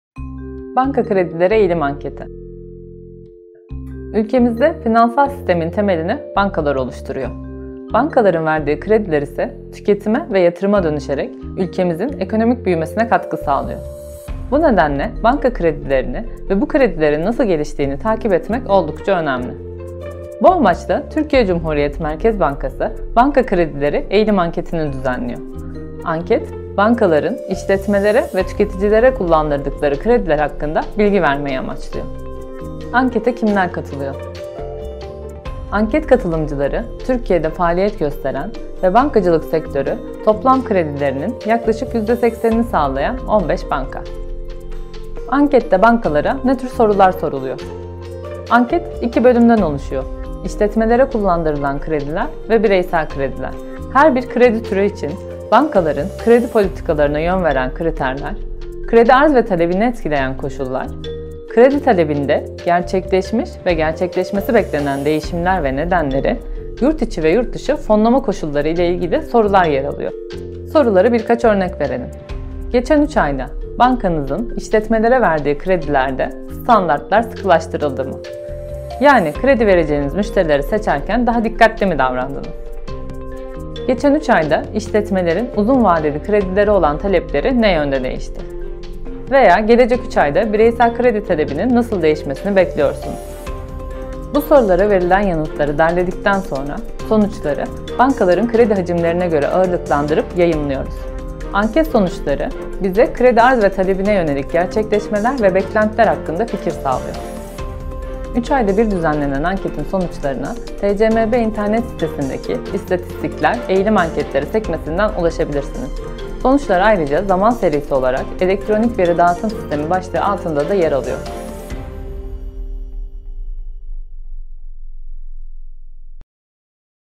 Banka Kredileri Eğilimi Anketi
Finansal sistemin temelini oluşturan bankaların verdiği krediler, tüketime ve yatırıma dönüşerek ekonomik büyümeye katkı sağlıyor. Bu nedenle, banka kredilerini ve bu kredilerin nasıl geliştiğini takip etmek oldukça önemli.